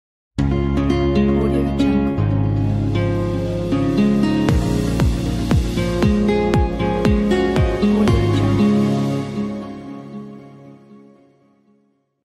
Black Paper Logo
Logo sting with a nice organic look and pre keyed footage. The template is very intuitive with simple controls and comments inside the proyect to guide you through the cusomization process. We hope you find it helpful